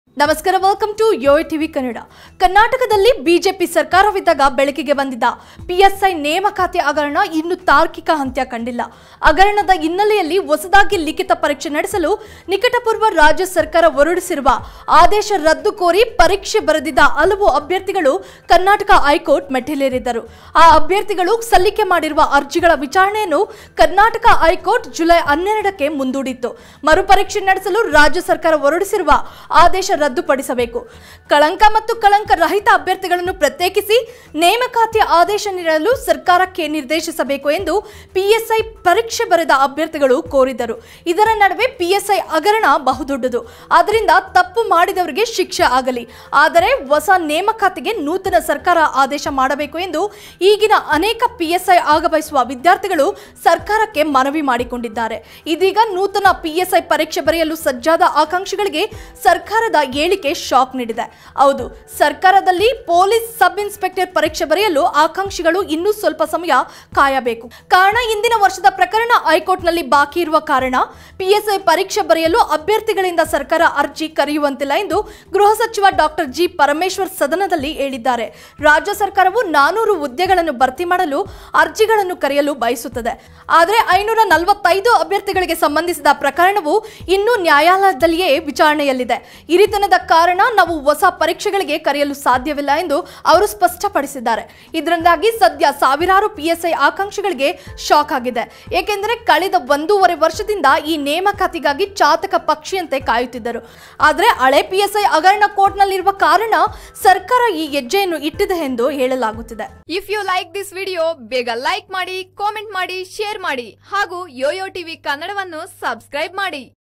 PSI ಆಗ ಬಯಸುವ ಆಕಾಂಕ್ಷಿಗಳಿಗೆ ಶಾಕ್ ! ಹೋಮ್ ಮಿನಿಸ್ಟರ್ ‌ಹೇಳಿದ್ದೇನು ? | PSI Karnataka | YOYO TV Kannada
ಕರ್ನಾಟಕದಲ್ಲಿ ಬಿಜೆಪಿ ಸರ್ಕಾರವಿದ್ದಾಗ ಬೆಳಕಿಗೆ ಬಂದಿದ್ದ ಪಿಎಸ್‌ಐ ನೇಮಕಾತಿ ಹಗರಣ ಇನ್ನೂ ತಾರ್ಕಿಕ ಅಂತ್ಯ ಕಂಡಿಲ್ಲ.

ಹಗರಣದ ಹಿನ್ನೆಲೆಯಲ್ಲಿ ಹೊಸದಾಗಿ ಲಿಖಿತ ಪರೀಕ್ಷೆ ನಡೆಸಲು ನಿಕಪೂಟರ್ವ ರಾಜ್ಯ ಸರ್ಕಾರ ಹೊರಡಿಸಿರುವ ಆದೇಶ ರದ್ದು ಕೋರಿ ಪರೀಕ್ಷೆ ಬರೆದಿದ್ದ ಹಲವು  ಅಭ್ಯರ್ಥಿಗಳು ಕರ್ನಾಟಕ ಹೈಕೋರ್ಟ್‌ ಮೆಟ್ಟಿಲೇರಿದ್ದರು.

ಈ ಅಭ್ಯರ್ಥಿಗಳು ಸಲ್ಲಿಕೆ ಮಾಡಿರುವ ಅರ್ಜಿಗಳ ವಿಚಾರಣೆಯನ್ನು ಕರ್ನಾಟಕ ಹೈಕೋರ್ಟ್ ಜುಲೈ 12ಕ್ಕೆ ಮುಂದೂಡಿತ್ತು. ಮರು ಪರೀಕ್ಷೆ ನಡೆಸಲು ರಾಜ್ಯ ಸರ್ಕಾರ ಹೊರಡಿಸಿರುವ ಆದೇಶ ರದ್ದುಪಡಿಸಬೇಕು.

ಕಳಂಕಿತ ಮತ್ತು ಕಳಂಕರಹಿತ ಅಭ್ಯರ್ಥಿಗಳನ್ನು ಪ್ರತ್ಯೇಕಿಸಿ, ನೇಮಕಾತಿ ಆದೇಶ ನೀಡಲು ಸರ್ಕಾರಕ್ಕೆ ನಿರ್ದೇಶಿಸಬೇಕು ಎಂದು ಪಿಎಸ್ಐ ಪರೀಕ್ಷೆ ಬರೆದ ಅಭ್ಯಾರ್ಥಿಗಳು ಕೋರಿದ್ದರು.